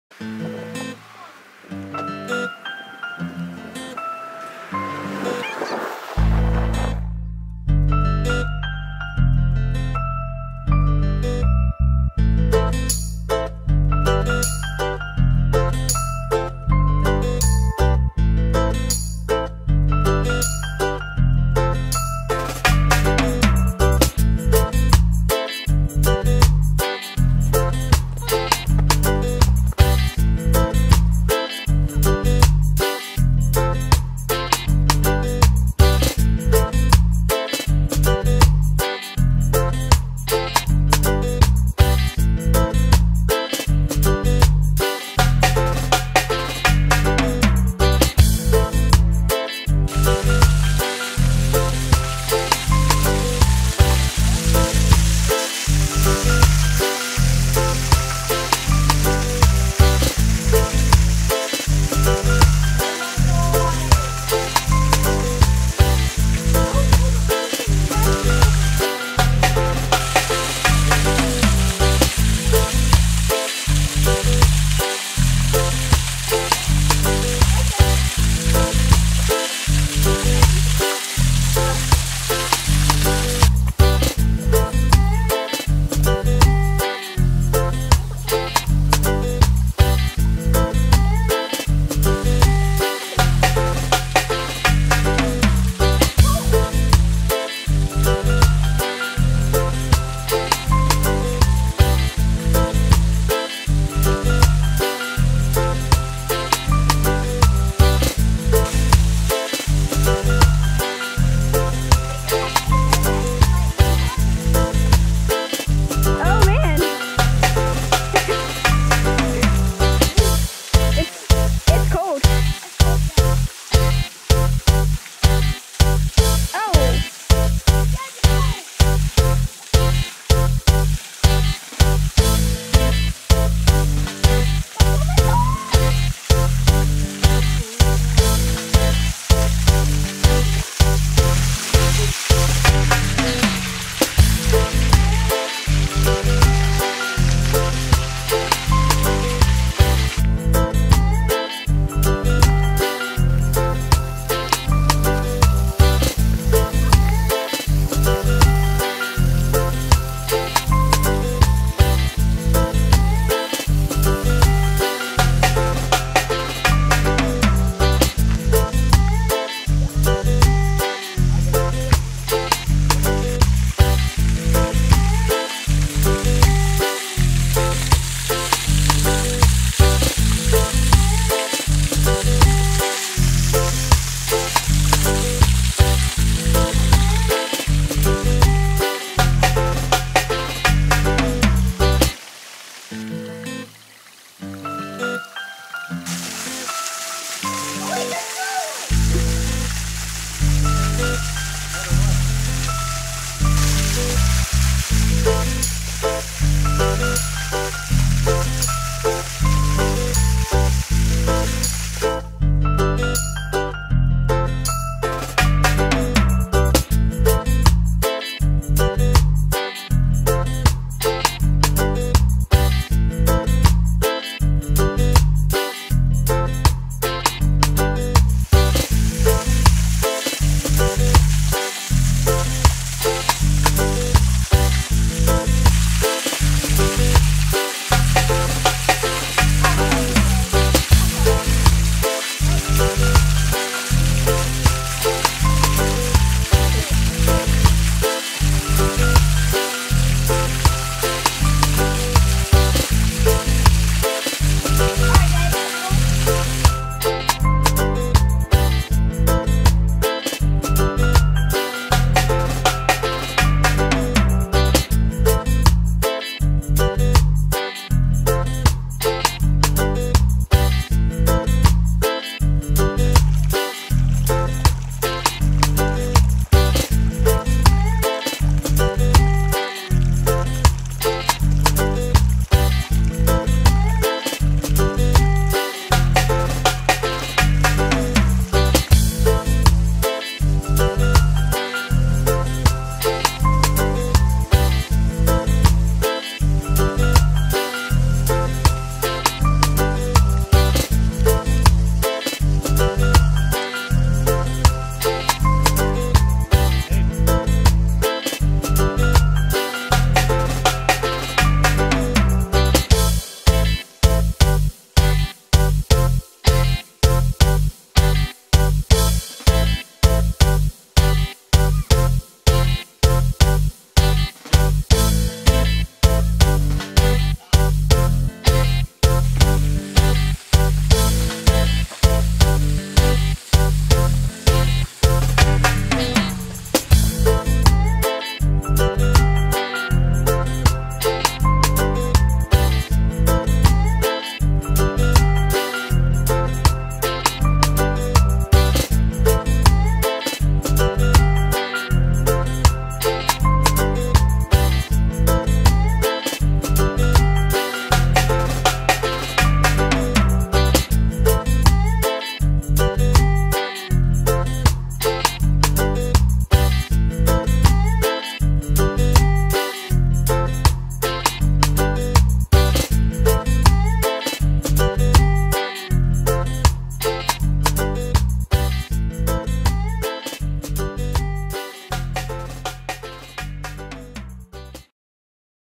Peach Tree Rock
Our adventures at Peach Tree Rock Heritage Preserve